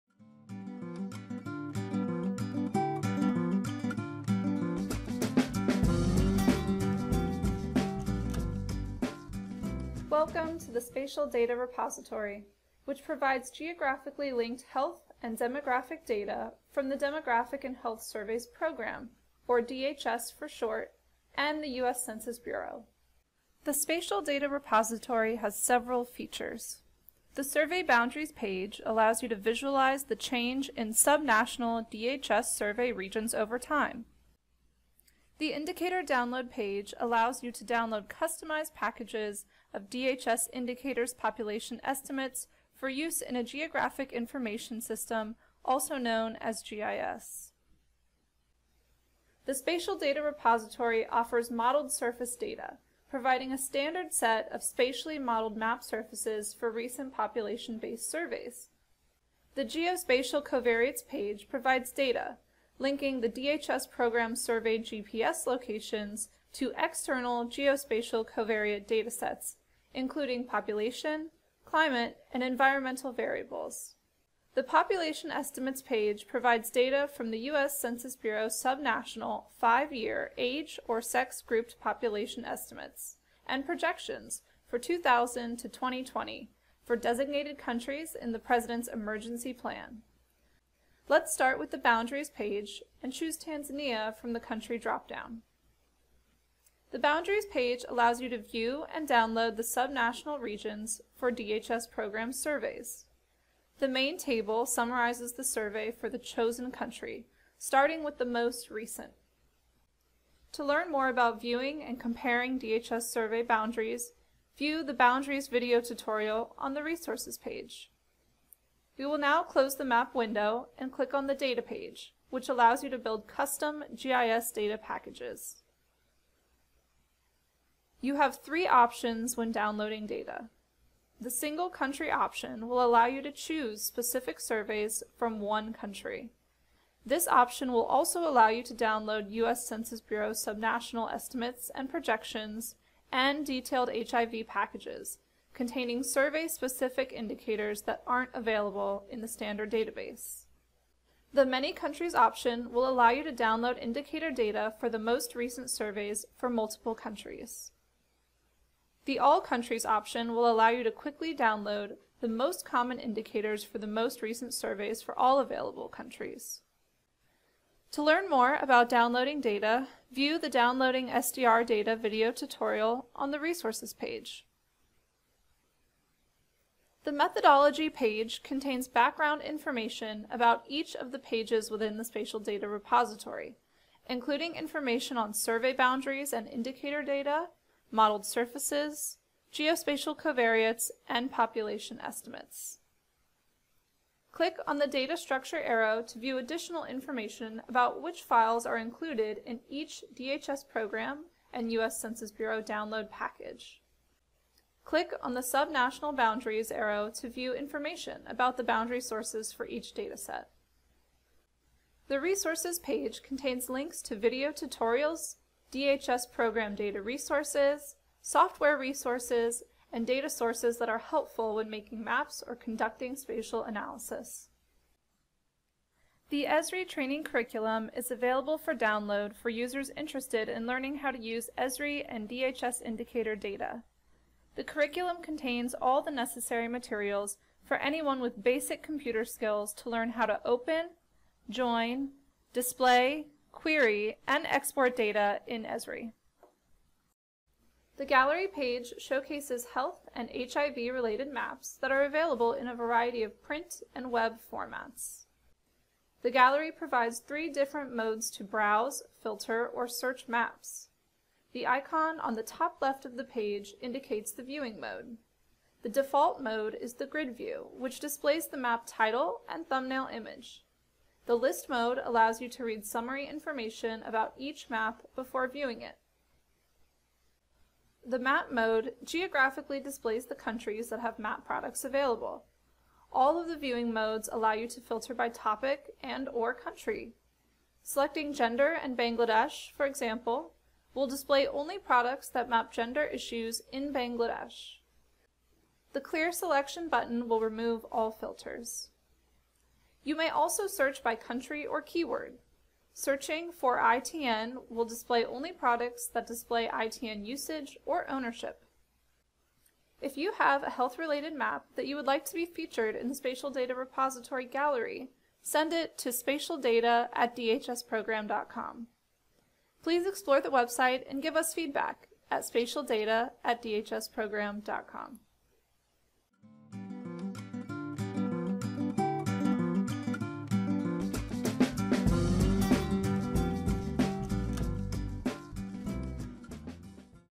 Introduction to the Spatial Data Repository
This video provides an overview of the Spatial Data Repository, which provides geographically-linked health and demographic linked data from The DHS Program.